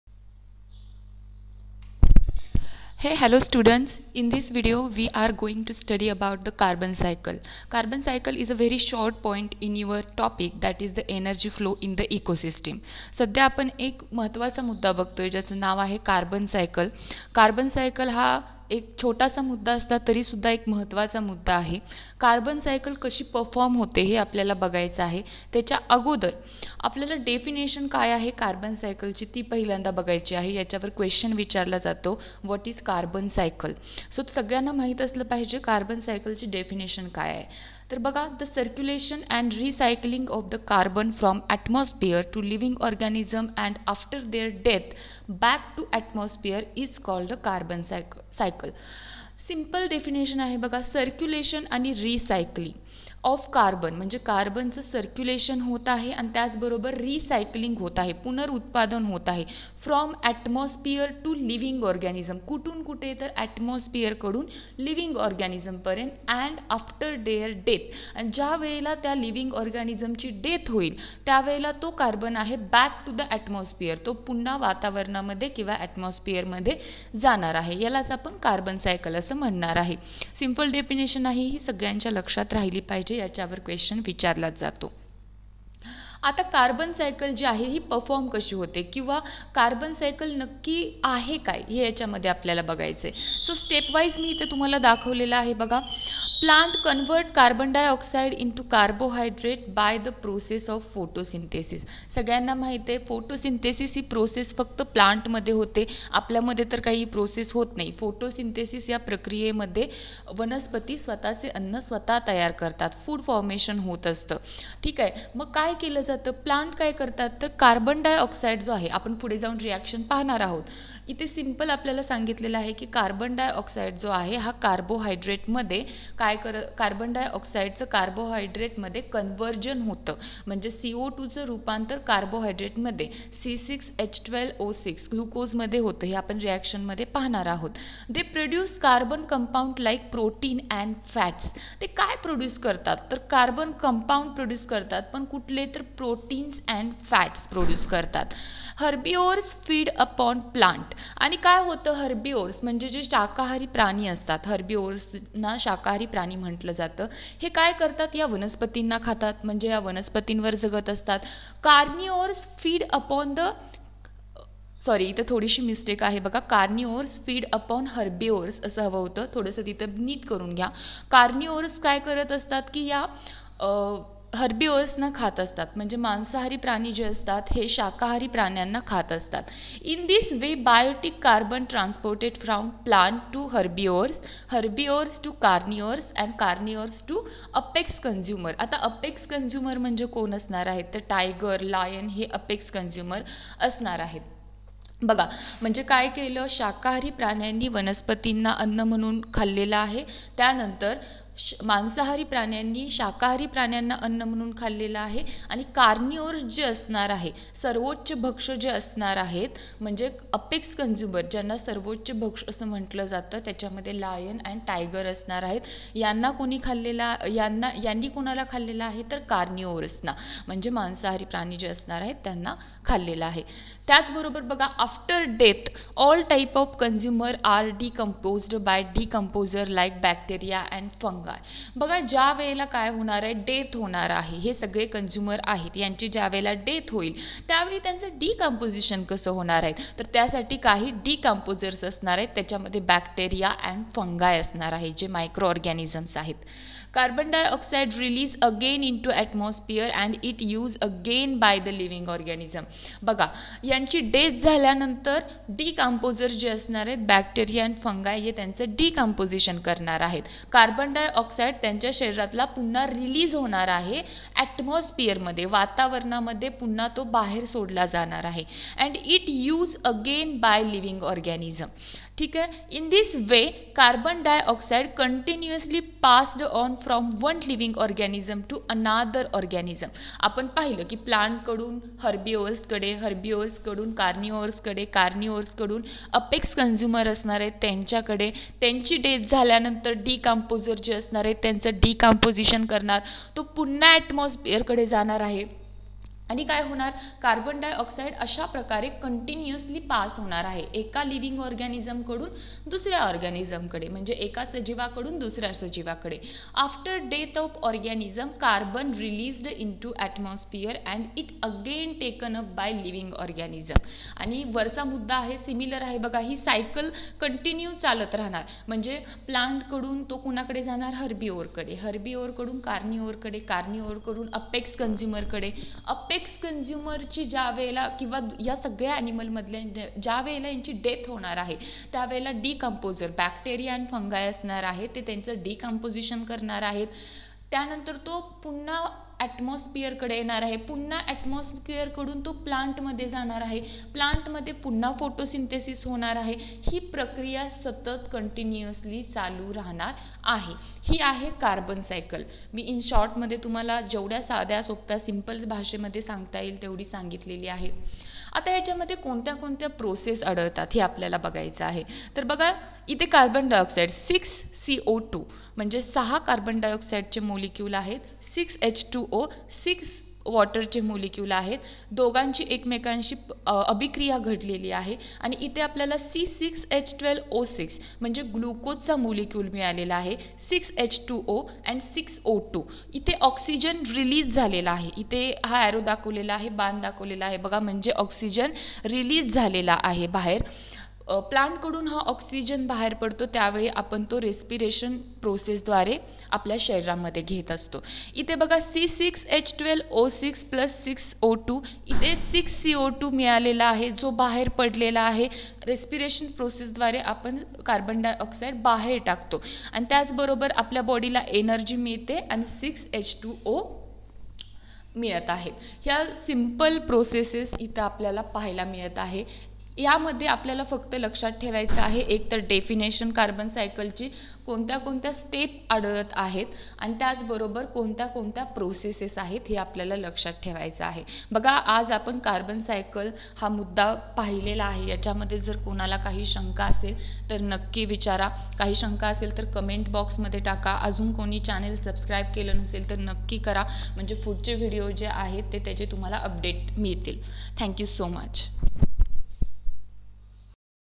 Carbon cycle (Energy Flow In Ecosystem)
Defination of carbon cycle
Steps occur in carbon cycle
processes of carbon cycle